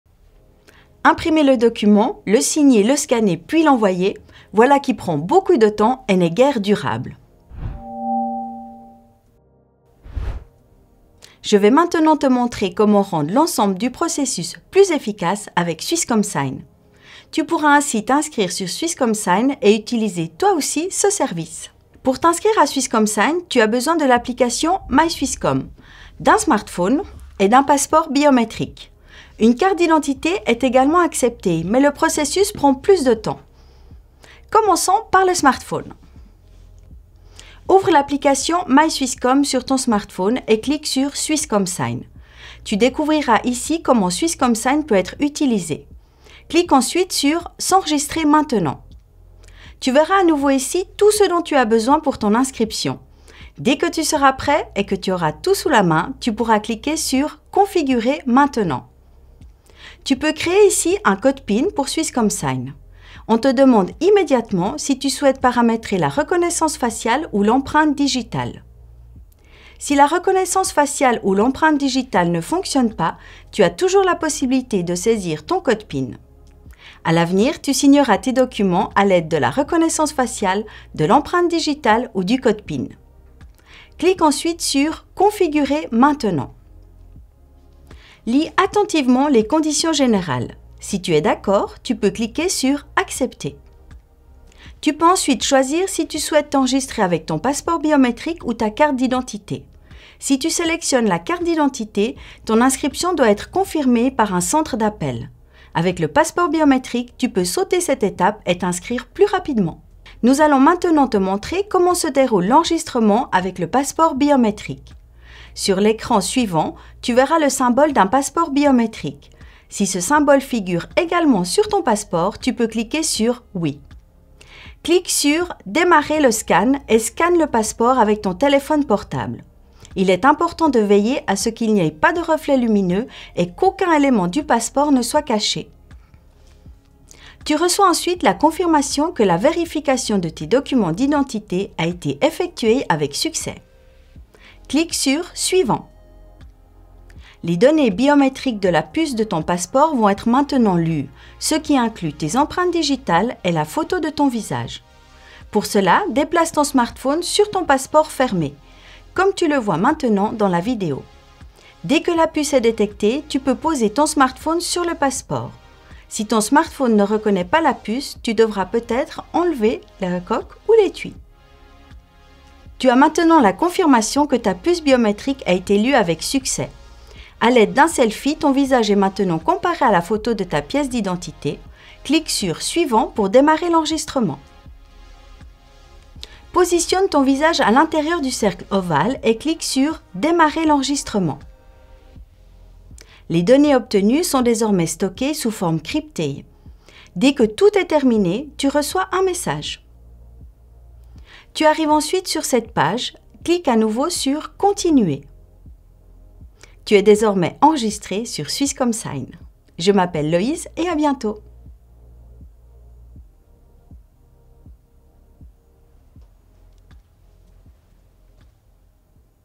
Inscription à Swisscom Sign - Swisscom Help
Dans cette vidéo, Loyse, formatrice chez Swisscom, te montre comment t'inscrire à Swisscom Sign.